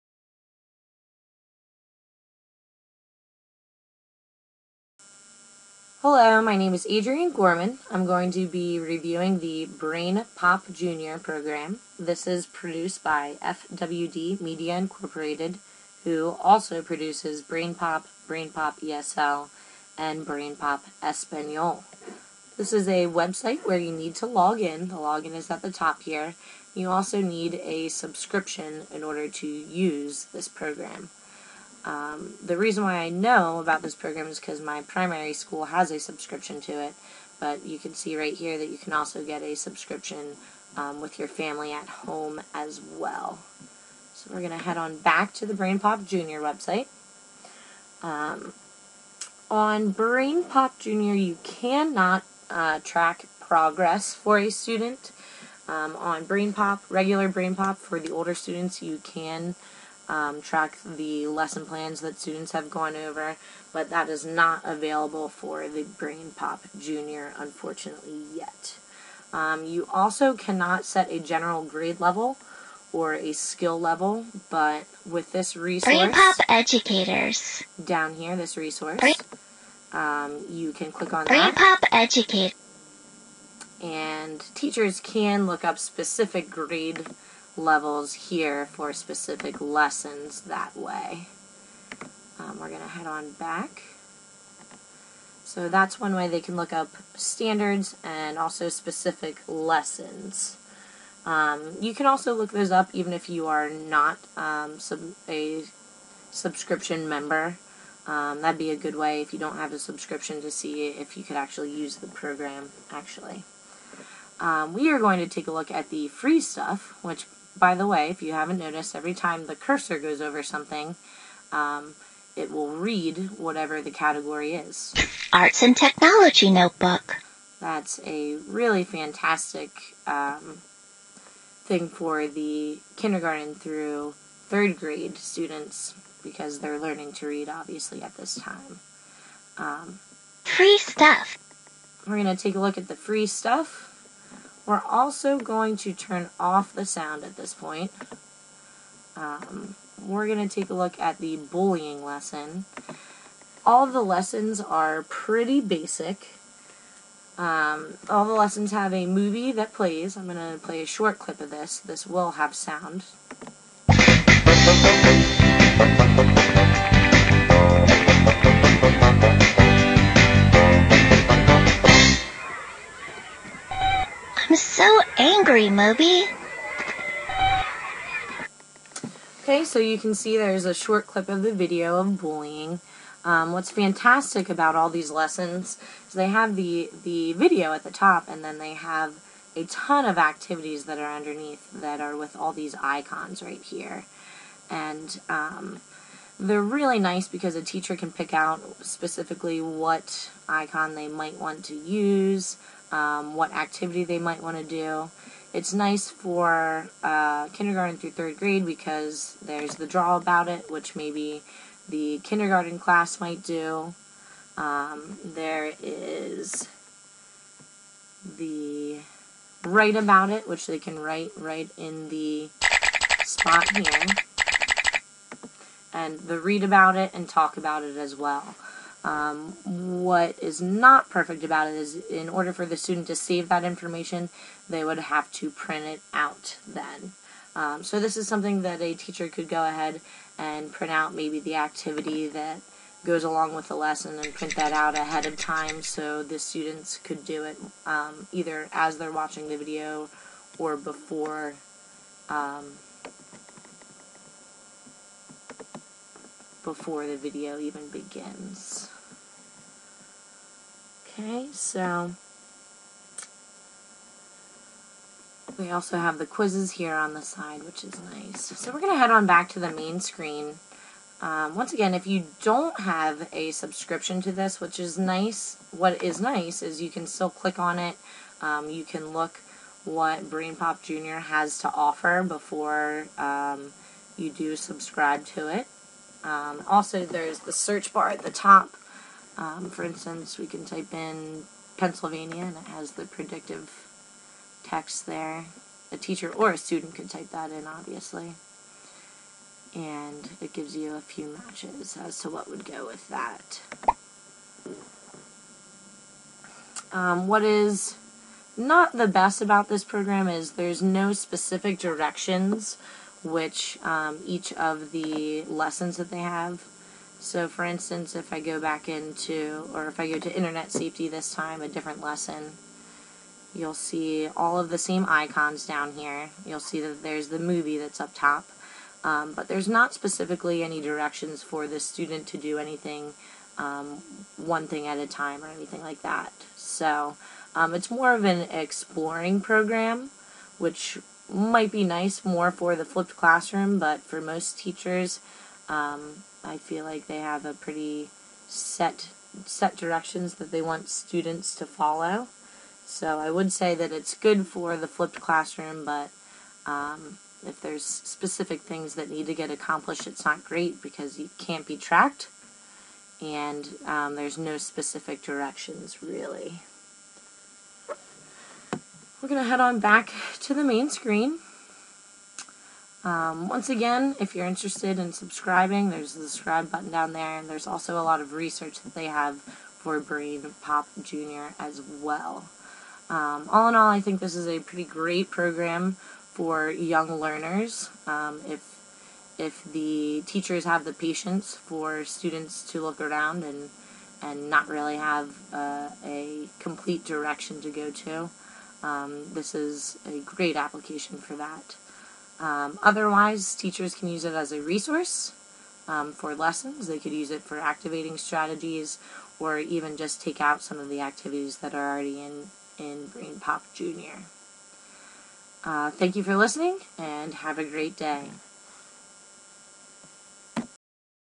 Brain Pop Jr. Review
This is a review of the educational program, _Brain Pop Jr._